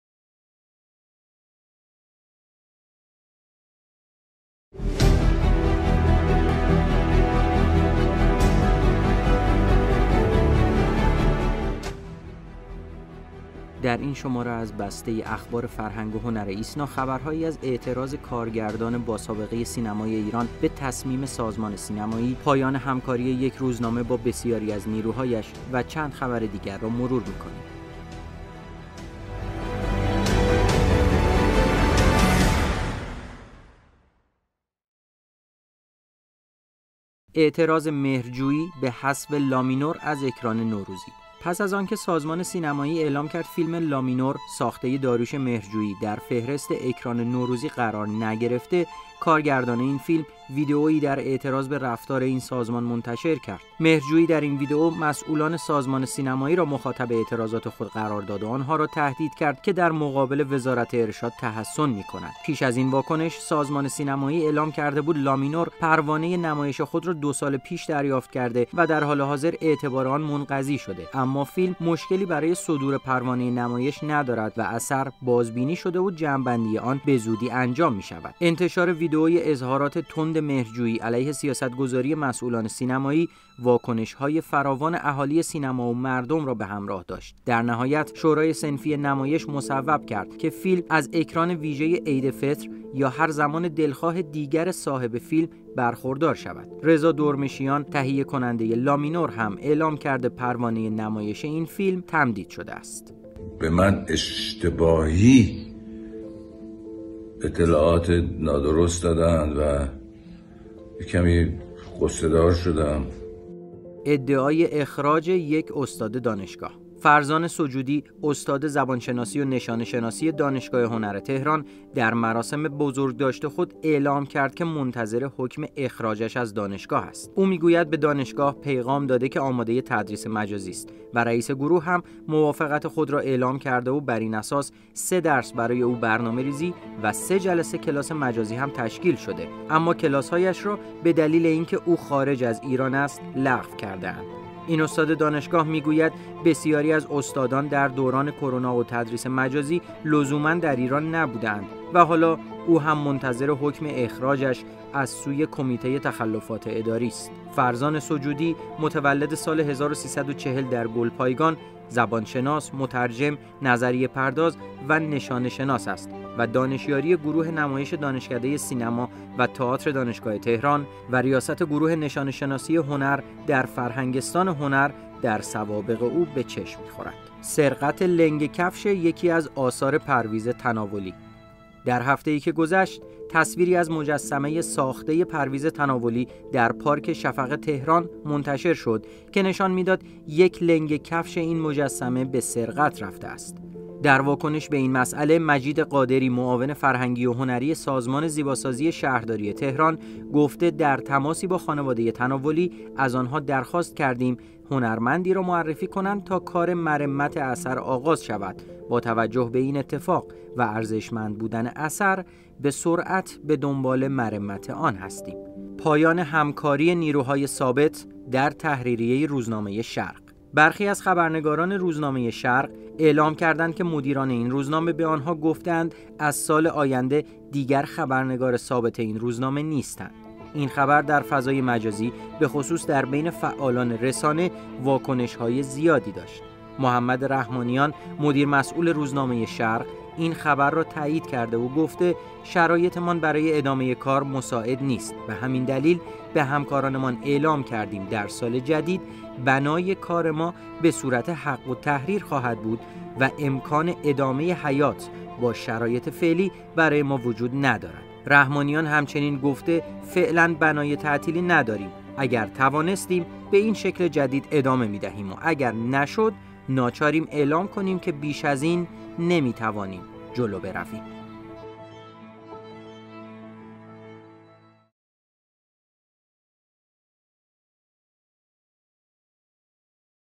از اعتراض مهرجویی تا ورشکستگی یک روزنامه
در این شماره از بسته اخبار فرهنگ و هنر ایسنا، خبرهایی از اعتراض کارگردانِ باسابقۀ سینمای ایران به تصمیم سازمان سینمایی، پایان همکاری یک روزنامه با بسیاری از نیروهایش و چند خبر دیگر را مرور می‌کنیم.